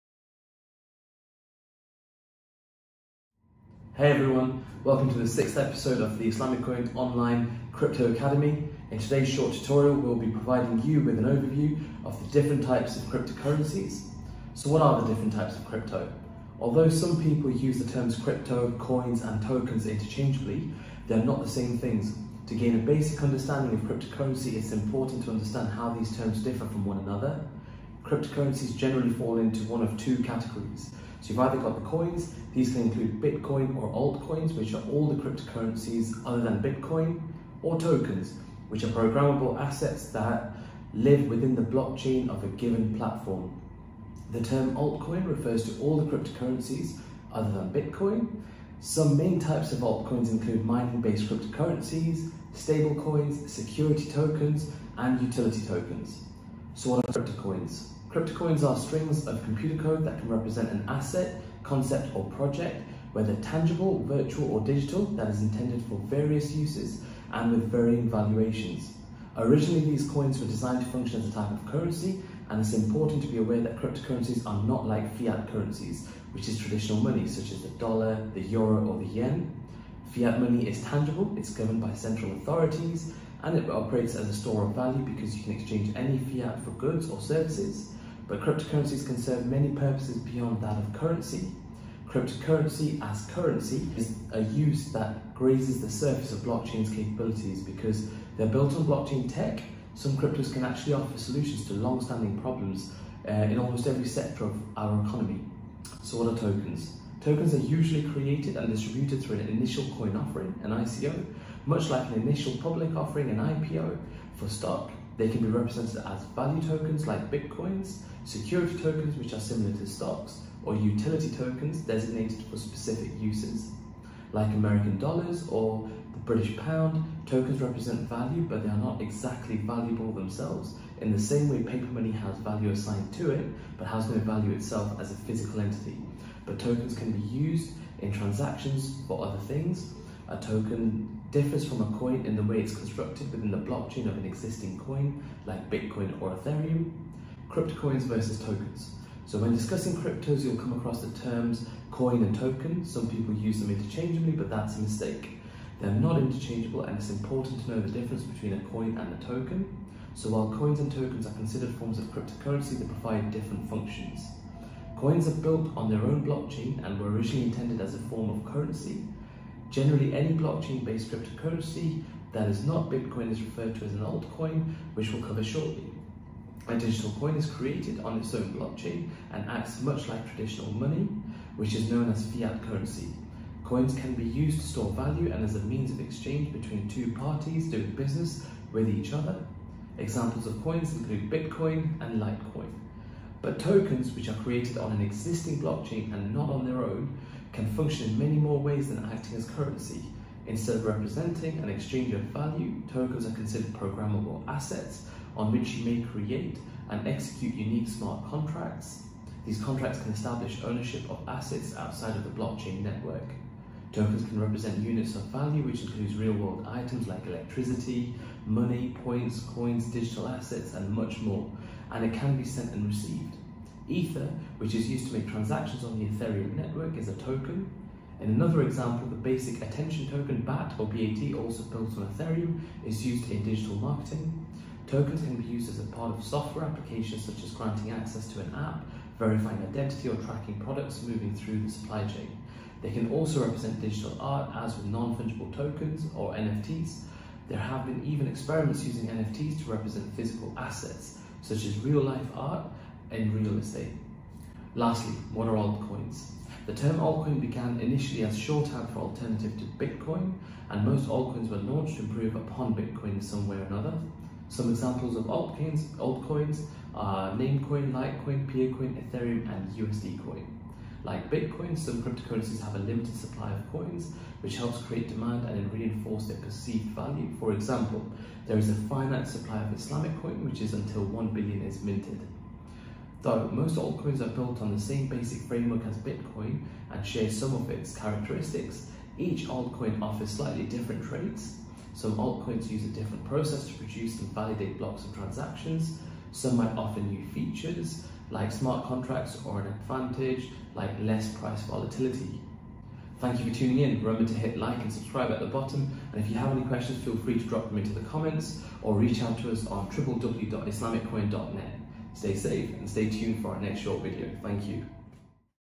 What are the different types of cryptos or cryptocurrencies? (For Beginners)
Although some people use the terms crypto, coins, and tokens interchangeably, they are not the same things. To gain a basic understanding of cryptocurrency, it’s important to understand how these terms differ from one another.

Cryptocurrencies generally fall into one of two categories:
•   Coins: Can include Bitcoin and Altcoins (all cryptocurrencies other than Bitcoin)
•   Tokens: Programmable assets that live within the blockchain of a given platform

The term altcoin refers to all cryptocurrencies other than Bitcoin. Some main types of altcoins include mining-based cryptocurrencies, stablecoins, security tokens, and utility tokens.